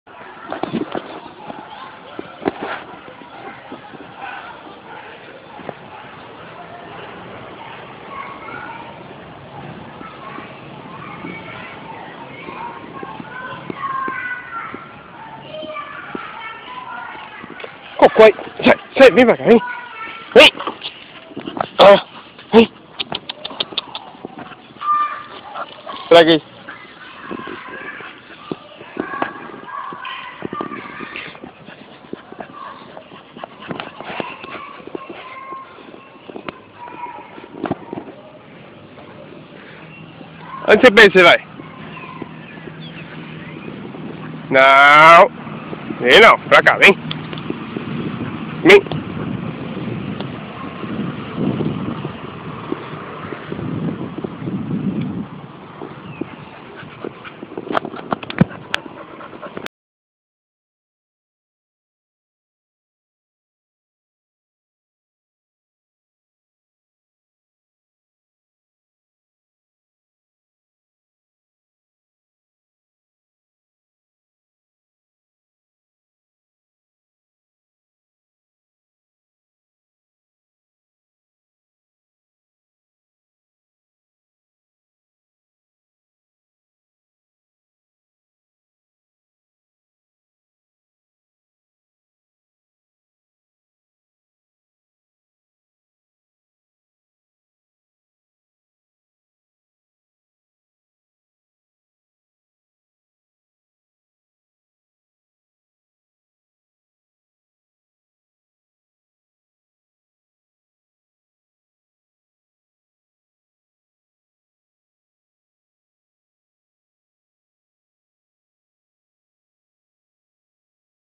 Bullterrier ( Diva passeando )
Diva dando um rolezinhoo no mato